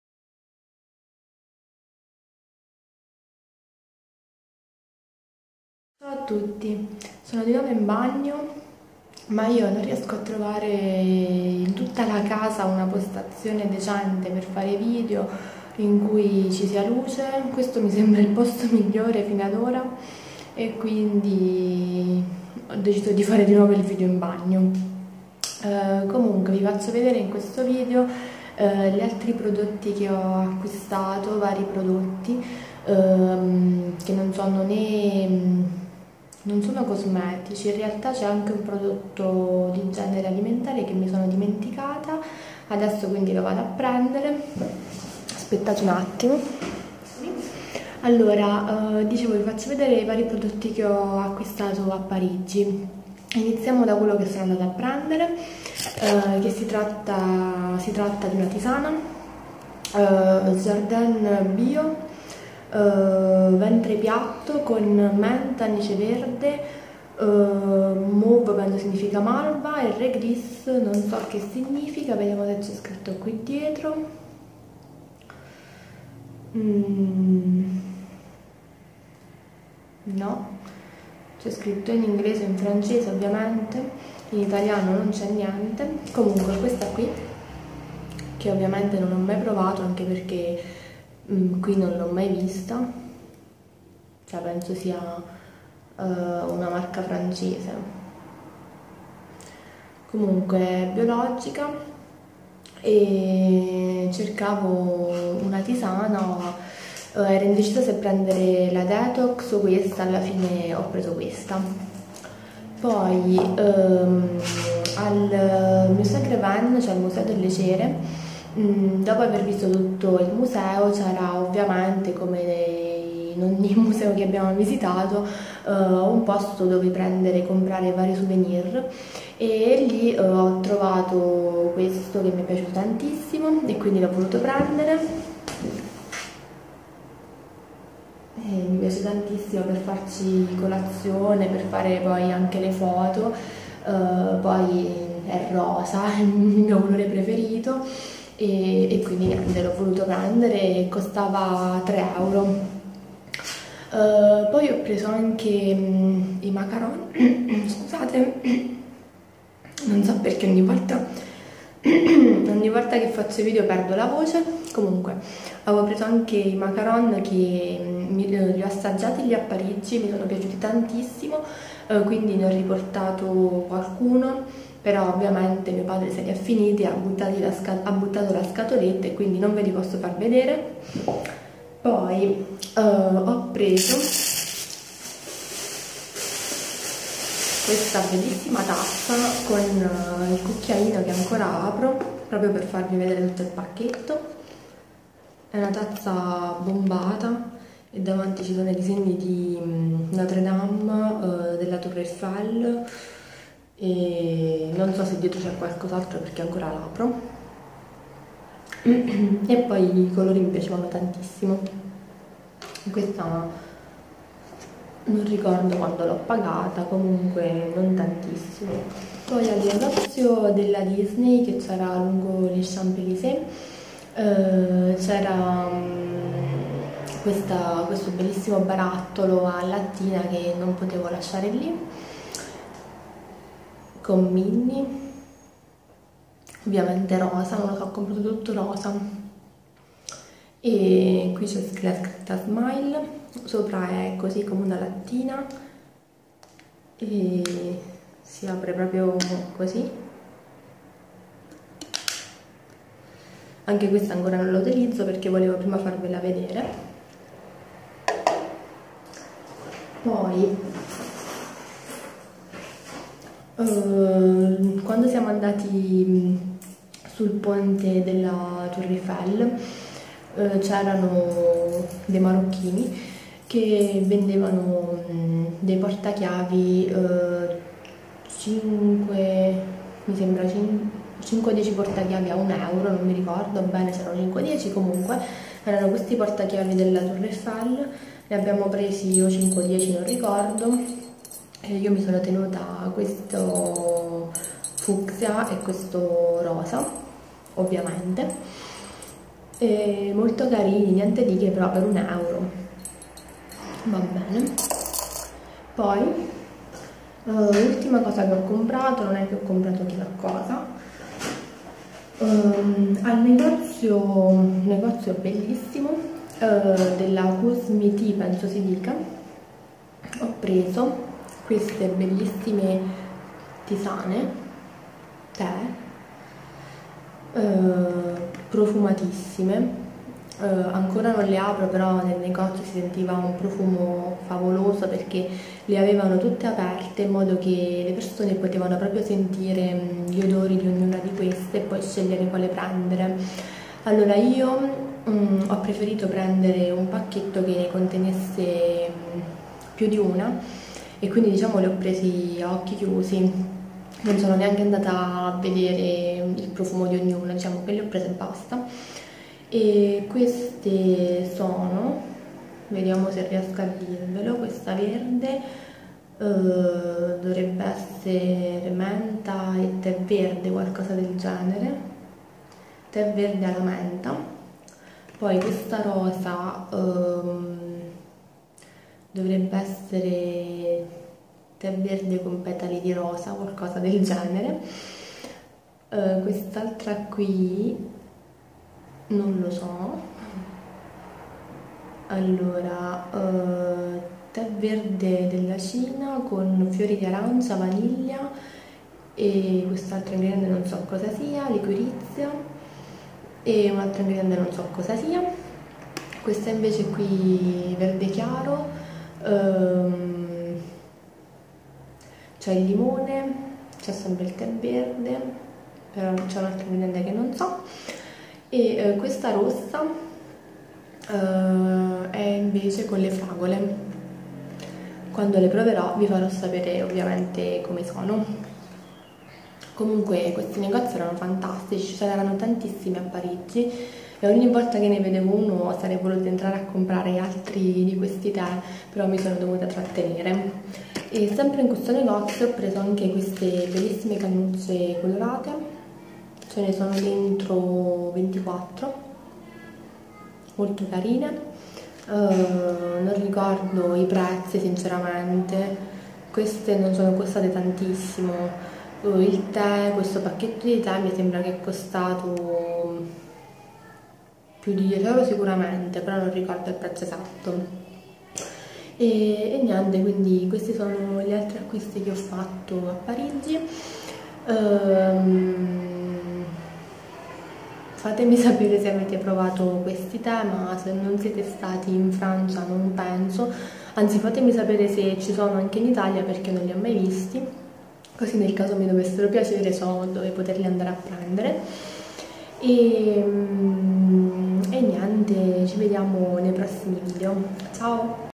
Acquisti a Parigi (seconda parte)/ Vari
Ecco la seconda parte degli acquisti fatti a Parigi!